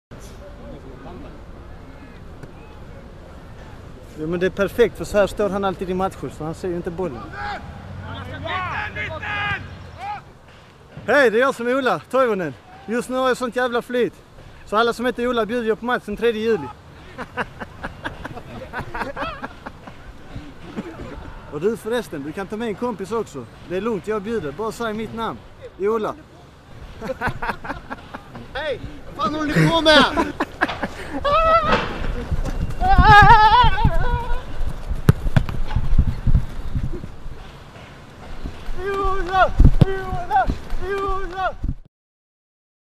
Malmö FF reklam inför MFF-Ljungskile 2008: Heter du Ola?
Labinot Harbuzi och Ola Toivonen.